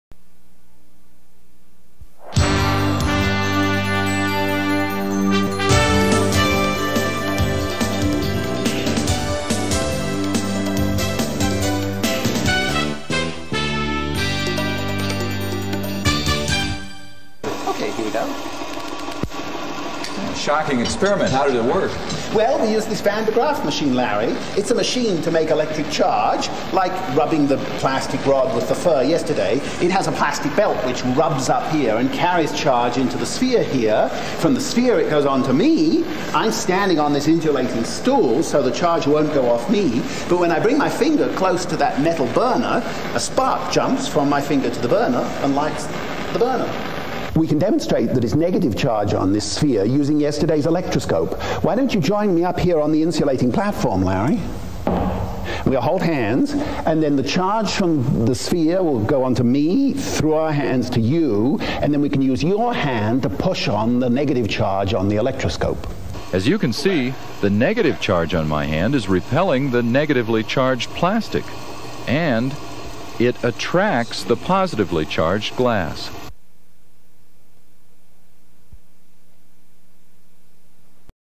2. More Static
Investigate the fundamentals of electricity such as charge, atoms, and energy.  Explore how electricity plays into lightning, circuits, and conductors, as well as how electricity can be used.  In this episode, learn about more about static.